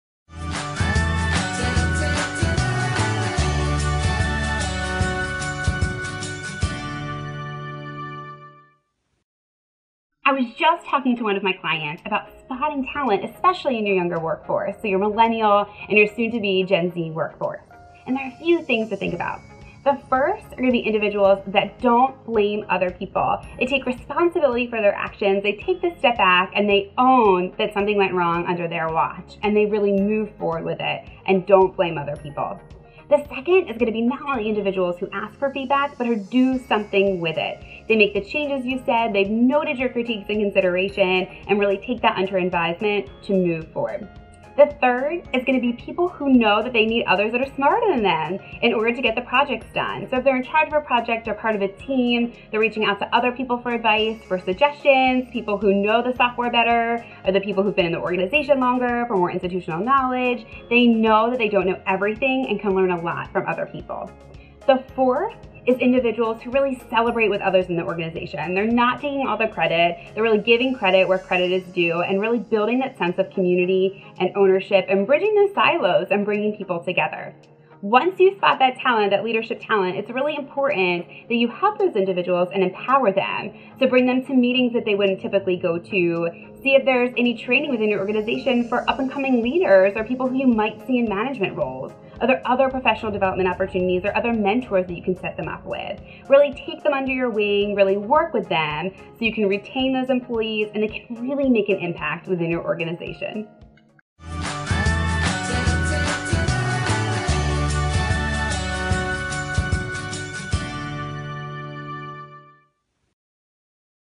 Spotting leadership talent
Learn how to spot talent in your millennial and gen z employees to know who to promote and when.